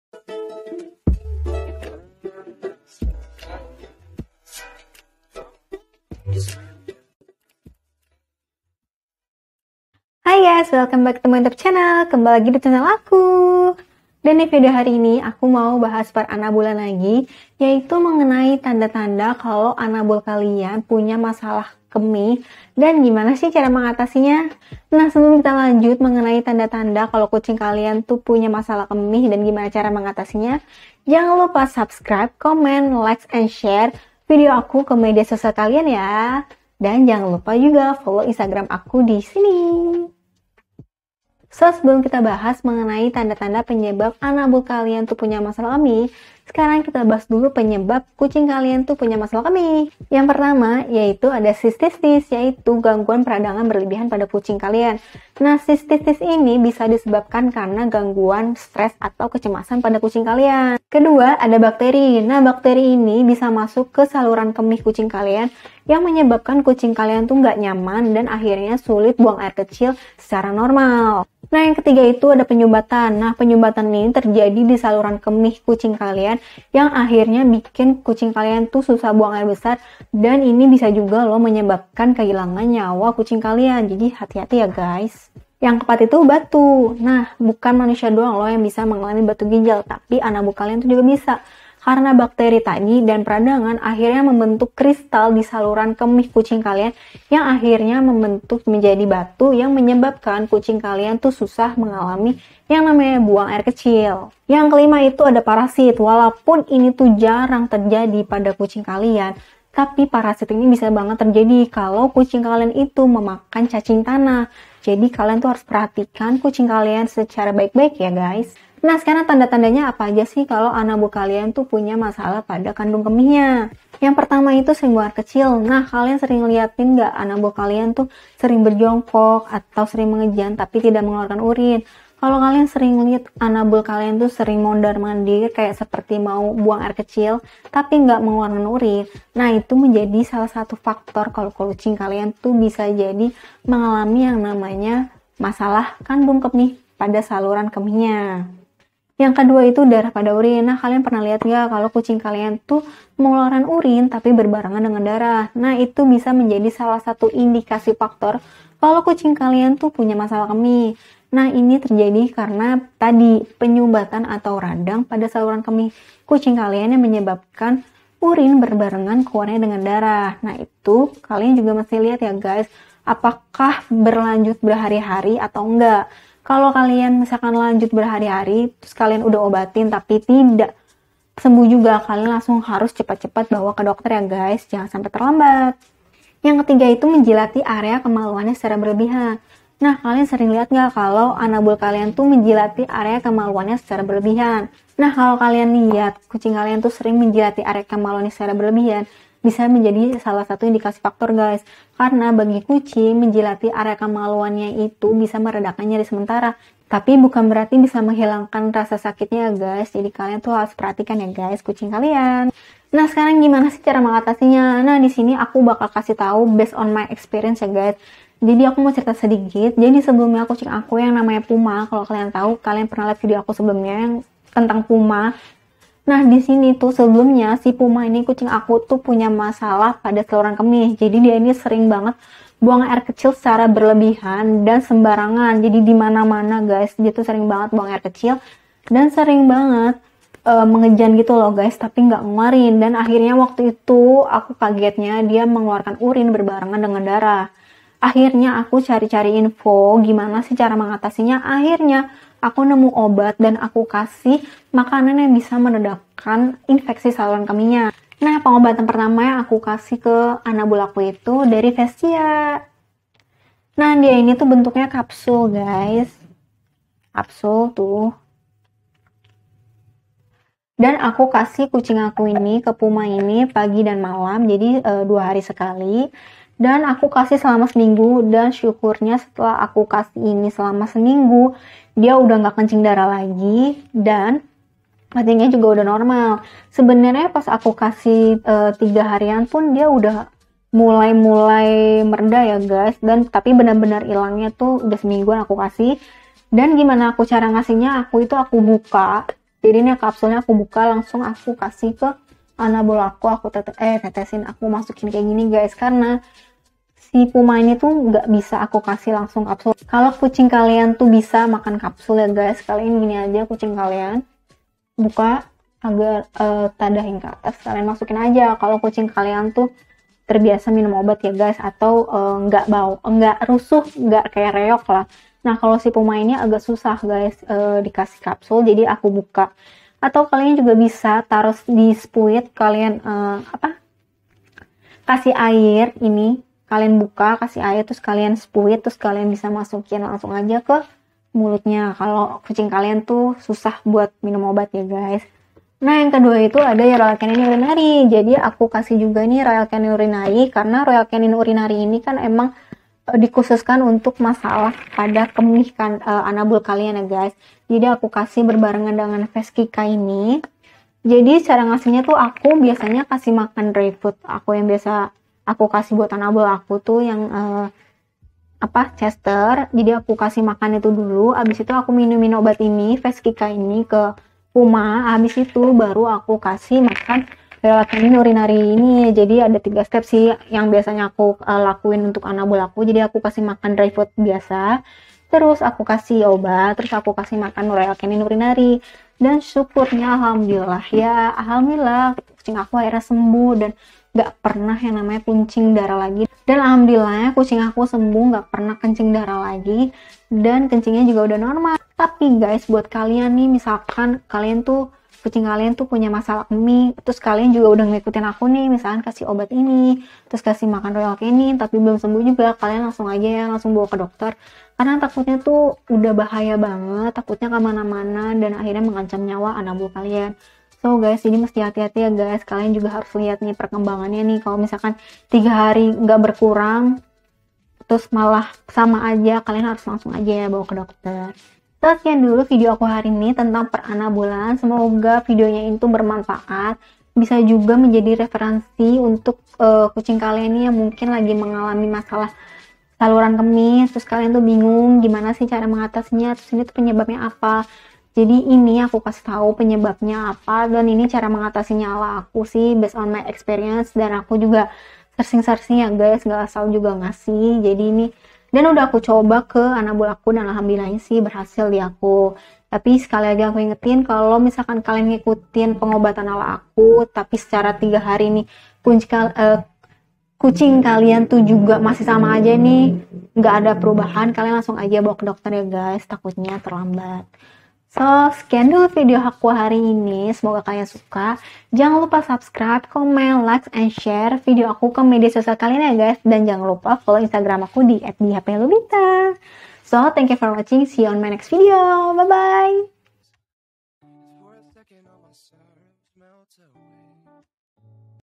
Penyebab kucing kencing darah & cara mengobatinya (pengalaman anabulku) | Dyah Lupita
Hai, hope you enjoy the video✨
Kalo anabul kita sakit kita pasti sedih bgt ya, apalagi kalo anabul kita sakitnya lumayan parah:( kayak pengalaman anabul ku yg pernah kencing darah saat itu. Dan alhamdulillahs sekarang sudah sembuh dan ini tips dari aku✨

what i use:
Camera: Canon EOS M100 H
Editing: inshoot
Thumbail: inshoot